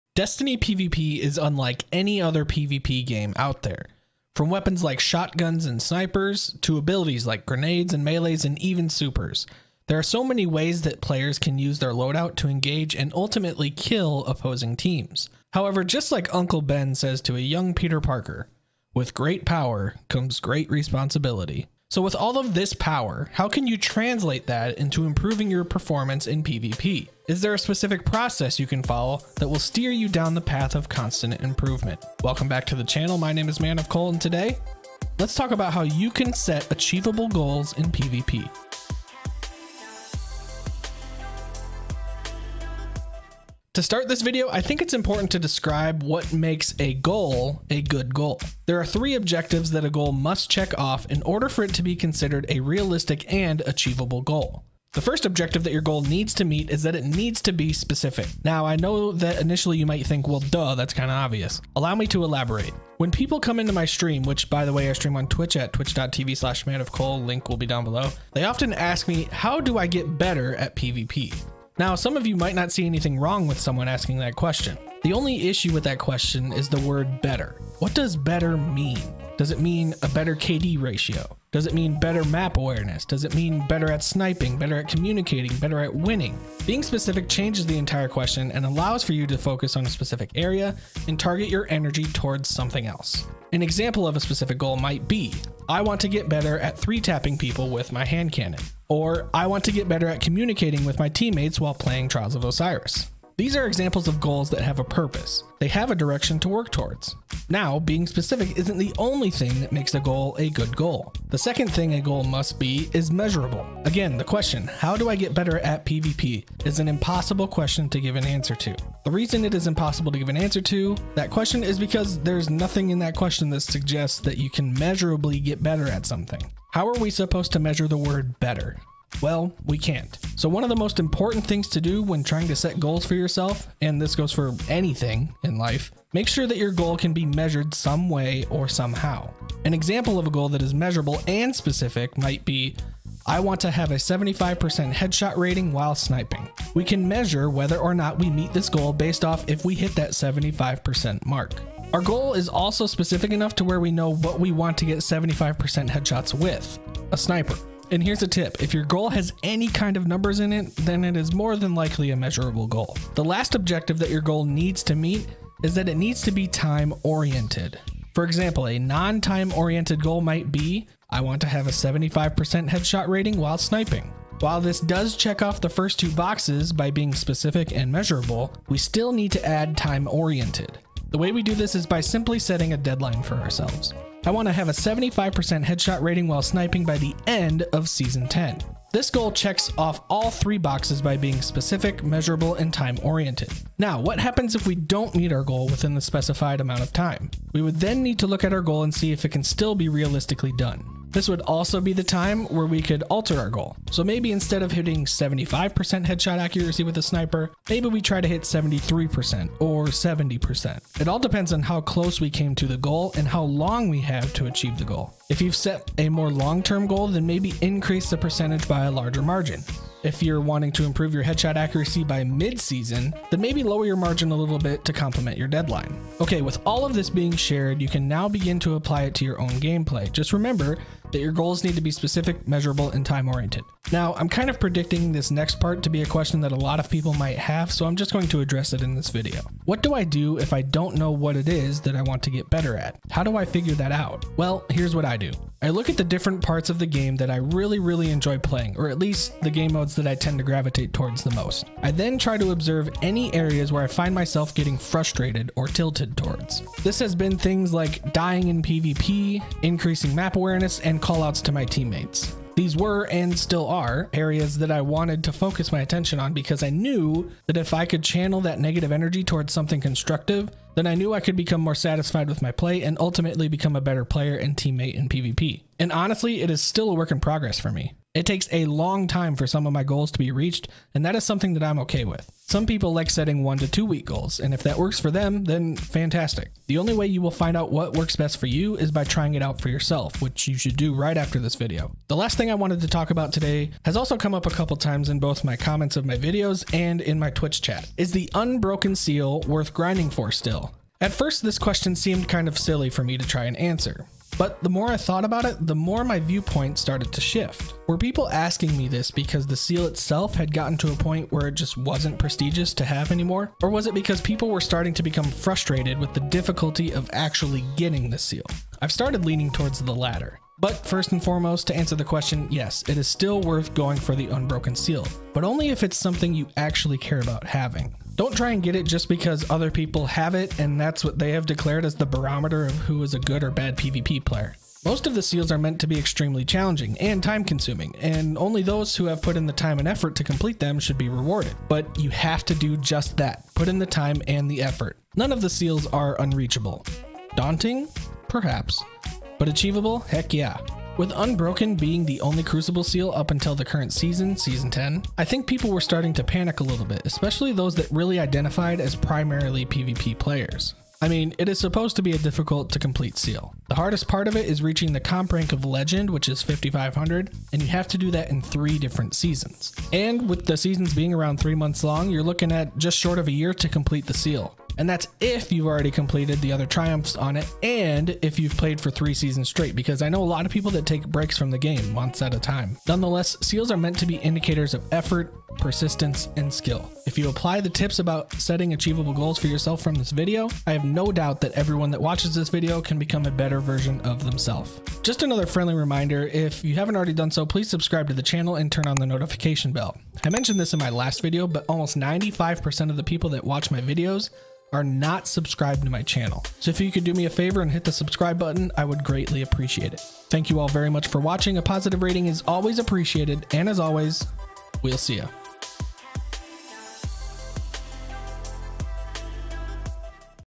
How to Set Achievable Goals in PvP | Creating Your Own Potential
In this video, I go over some different strategies to help you set and achieve goals to constantly improve in PvP. I also talk about the significance of the Unbroken Seal within the games current state. Links to socials down below.